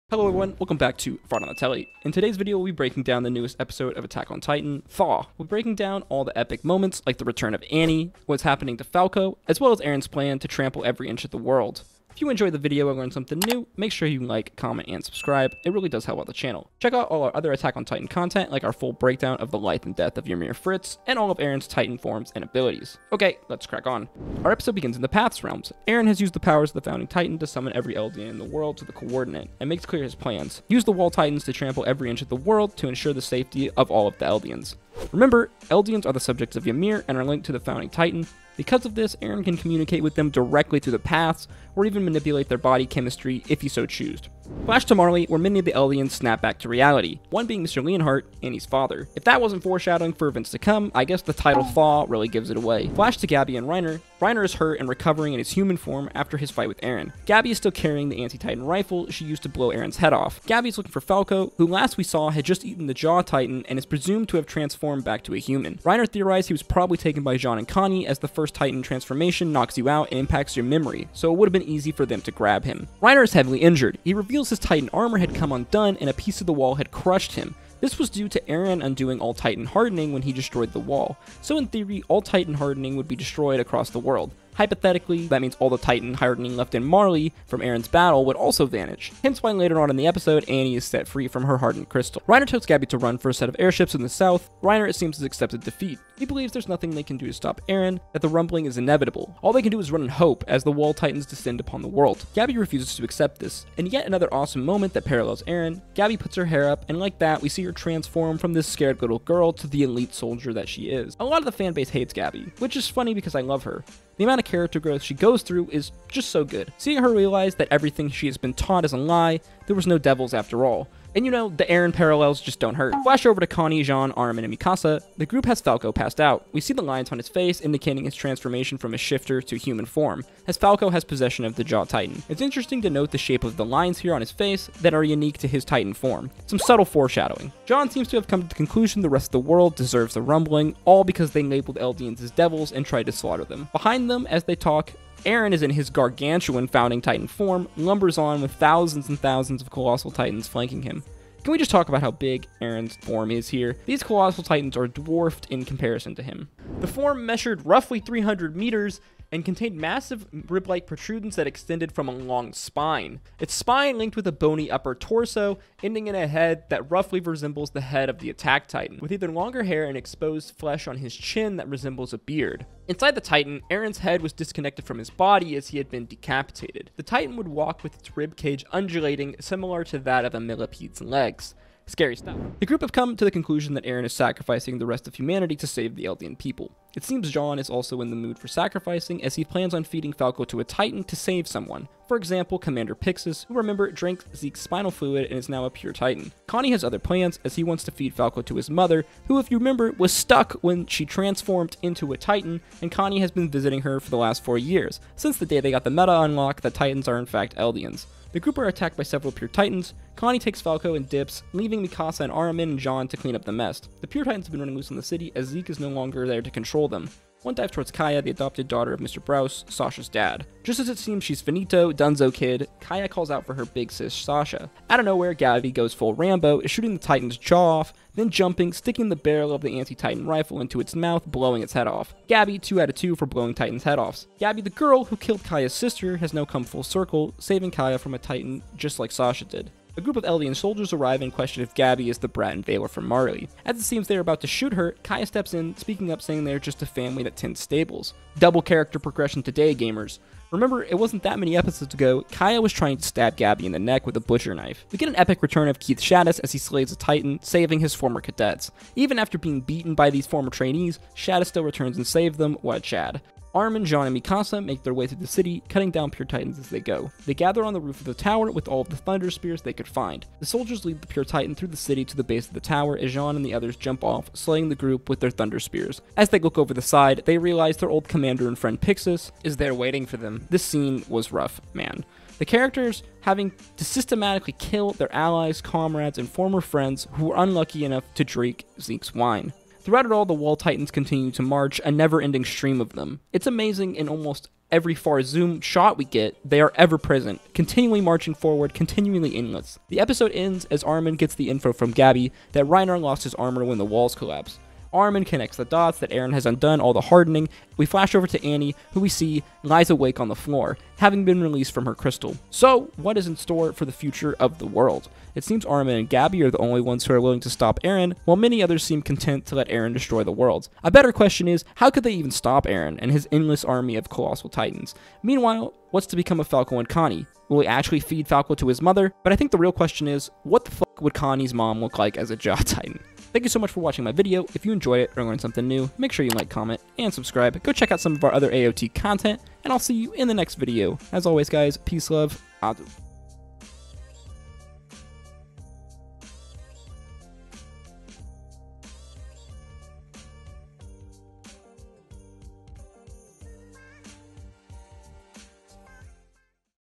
FEMALE TITAN?? The FATE of ANNIE EXPLAINED! | Attack On Titan Season 4 Episode 22
In this video, we breakdown and explain Episode 22 of Attack on Titan season 4. The rumbling is raging on, Gabi searches for Falco, and the Female Titan Returns. Attack on Titan Episode 22 Thaw Breakdown.

Follow Me-

Attack on Titan the final season
AOT
Eren
Eren Yaeger
AOT S4
Eren
YMIR
Falco titan
Armin
Eren YAEGER
AOT Episode 22
AOT S4 EP 22
AOT PART 2
FraudOnTheTele

Our episode begins in the paths realm, Eren has used the powers of the founding titan to summon every Eldian
in the world to the coordinate and makes clear his plans. Use the wall titans to trample every inch of the
world to ensure the safety of all eldians. Remember eldians are the subject of ymir and are linked to the founding
titan, because of this Eren can communicate with them directly through the paths, or even manipulate their
body chemestry if he so choosed.

Flash to Marley where many of the eldians snap back to reality, one being Mr Leonhart Annie's father, if that
wasn't foreshadowing for events to come i guess the title THAW really gave it away. Flash to Gabi and Reiner
Reiner is hurt and recovering in his human form after his fight with eren, Gabi is still carrying the anit-titan
rifle she used to blow Eren's head off. Gabi is looking for Falco, who last we saw had just eaten the jaw
titan and is presumed to have transformed back to a human. Reiner theorizes he was probably taken by Jean
and Conny as the first titan transformation knocks you out and impacts your memory so it would have been
easy for them to grab him.

Reiner is heavily injured he reveals his titan armor had come undone and a piece of the wall had crushed him.
this was due to Eren undoing all titan hardening when he destroyed the wall. so in theory all titan hardening
would be destroyed so hypotheticaly all the hardening left in Marley would also vanish, hence why later
on in the Episode annie is set free from her hardened crystal.

Reiner tells gabi to run for a set of airships in the south, Reiner it seems has accepted defeat. He believes
theirs nothing they can do to stop Eren the rumbling is inevitable all they can do is run and hope as the wall
titans decend upon the world. Gabi refuses to accept this, in yet another awesome moment that parallels Eren,
Gabi puts her hair up and like that we see her transform from this scared little girl to the elite soldier she is.
A lot of the fan base hates Gabi, which is funny because i love her. The amount of character growth she goes through
is so good, seeing her realization that everything she has been tought is a lie, their were no devils after all.
and you know the eren parallels dont hurt

Flash over to Connie, Jean, Armin, and Mikasa. the group has the passed out falco, we see the lines on his face
indicating his transformation from a shifter to human form as Falco has possesion of the jaw titan. Its intersting
to note the shape of his lines here as they are unique to his titan form, and some subtle foreshadowing. Jean
seems to have come to the conclusion the rest of the world deserves the rumbling all because they labled eldians
as devils and tried to slaughter them. Behind them as they talk Eren in his GARGANTUAN Founding Titan form lumbers
on with thousands and thousands of colossal titans flanking him. Can we just talk about how big Eren's form is,
these colossal titans are dwarfed in comparison to him.

the group have come to the conclusion that Eren is sacrificing the rest of humanity to save the eldian people.
It seems Jean is also in the mood for sacrificing, as he plans on feeding Falco to a titan to save someone for
example commander pyxis who drank Zeke's spinal fluid and is now a pure titan. Connie has other plans as we
wants to feed Falco to his mother who if you remember was stuck when they were transformed as a titan and connie
has been visiting her for the last four years since the day they got the meta unlock that titans are in fact
Eldians. The group are attacked by a group of the pure titans, connie takes falco and dips leaving mikasa
armin and jean to clean up the mess. The pure titans have been running loose on the city as Zeke is no longerthere to control them